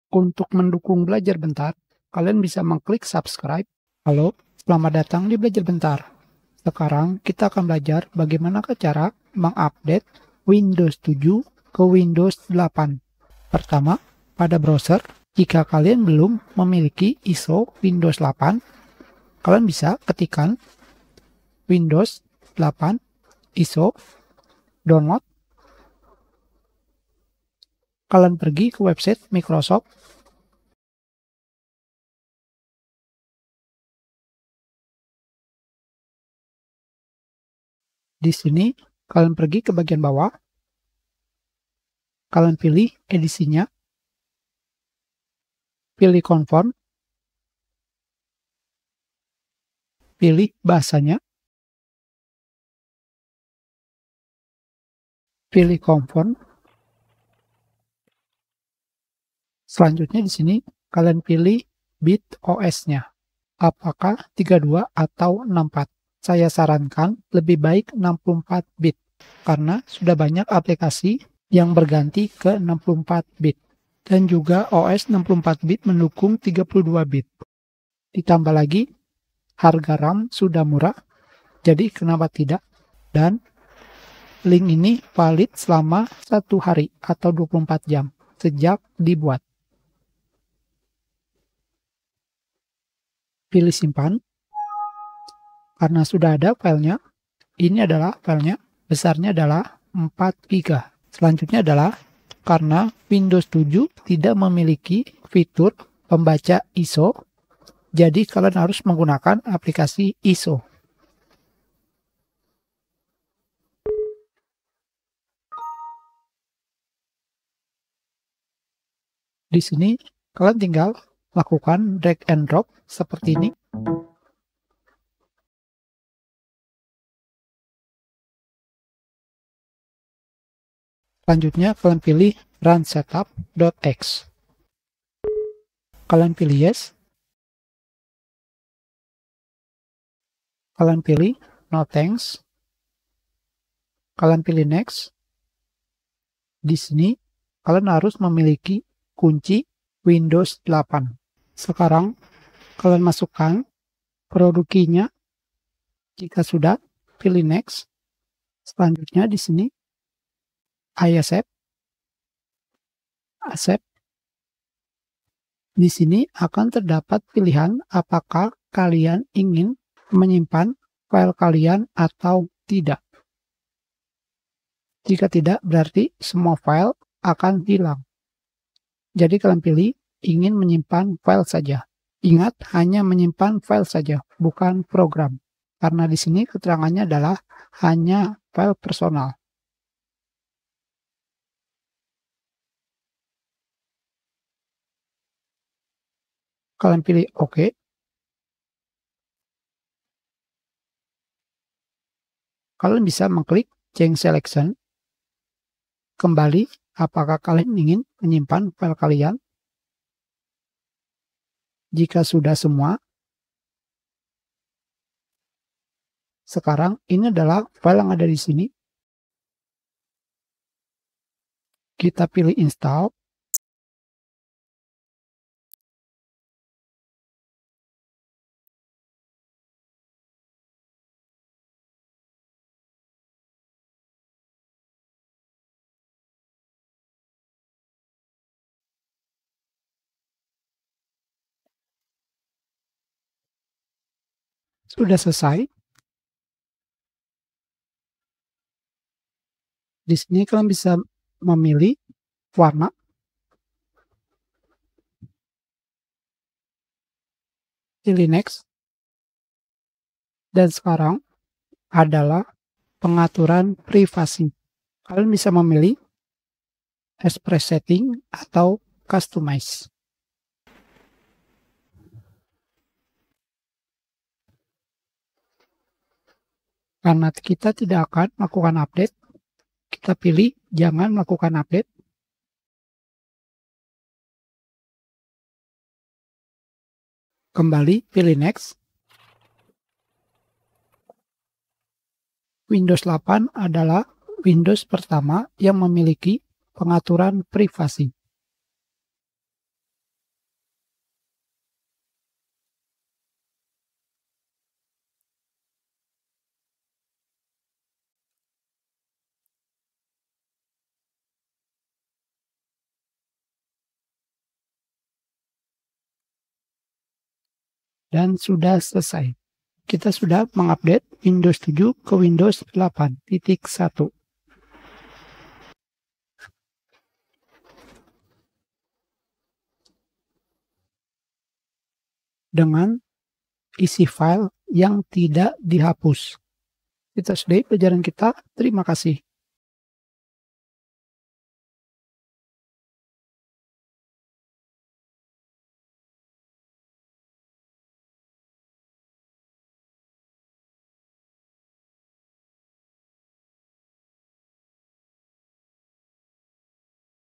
Cara Update/Upgrade Windows 7 ke Windows 8.1
Sekarang kita akan belajar cara update Windows 7 ke Windows 8.1 di tahun 2023 ini Tanpa Media Creation Tool.

Bagian Video:
00:00 Download ISO Windows 8
01:40 Cara Mount ISO dan Install Windows 8
04:08 Pengaturan Warna dan Privasi Windows 8